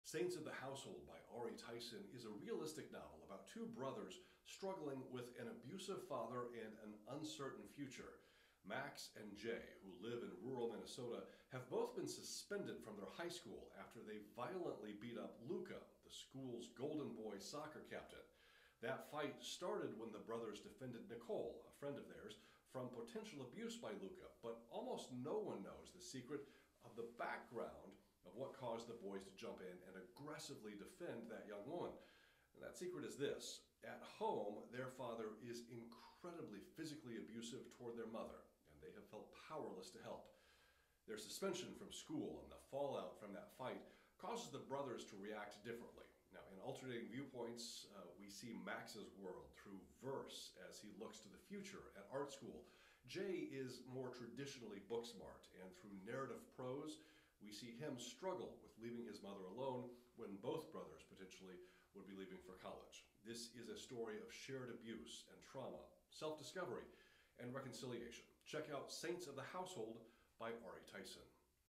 Saints of the Household by Ari Tison
A high school librarian reviews the young adult novel Saints of the Household by Ari Tison in a 60ish second book talk with no spoilers. Tison and the two main characters in the book are BriBri: indigenous Costa Rican.